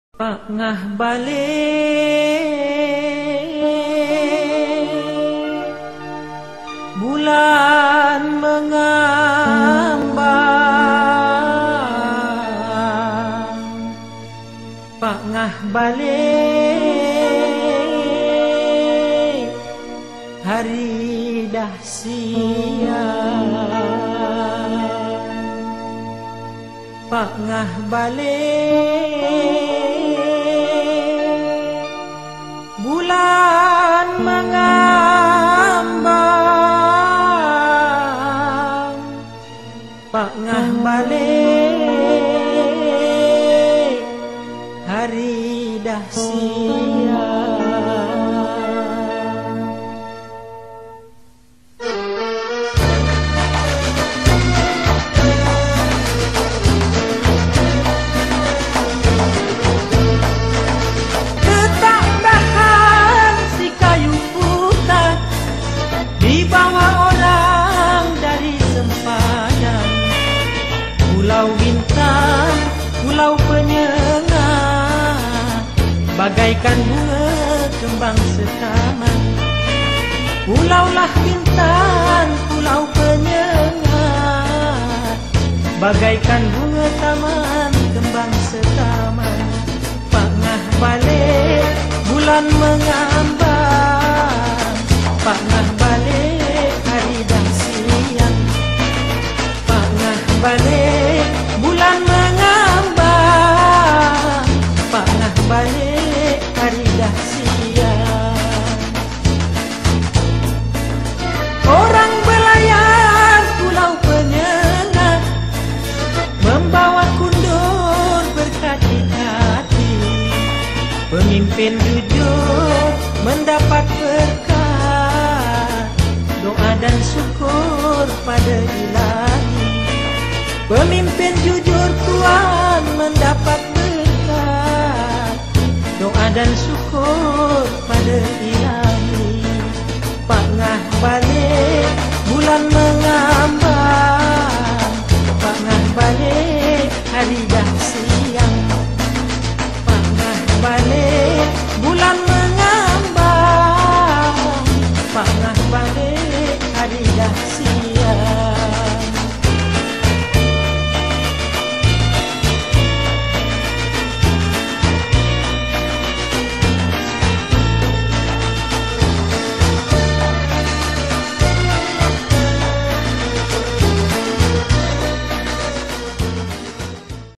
Gerak Tari Kreasi - Adik Wan Muhamad Irfan Naufal
Rinduku Si Bulan Mengambang - "la,aki tunggu tapi ada". Seperti menunggu Pak Ngah balik Bulan Mengambang, begitu juga aku menunggumu pulang ke pangkuanku. Cahayamu bak pelita sebagai perlambangan yang menerangi hidupku. Mengangkat pola yang teransang daripada perubahan bentuk fasa-fasa bulan, rindu menunggu kemunculan bulan mengambang yang menerangi di malam hari. Terinspirasi daripada Zapin Mahpom, gerak tari kreasi diangkat dalam karya ini tanpa meninggalkan ragam wajib zapin seperti taksim, kopak dan wainab.
- Bimbingan oleh Cikgu Sahril Ahmad (Penasihat Kelab Kaki Tari Anak Koyan, SK Long Gang.)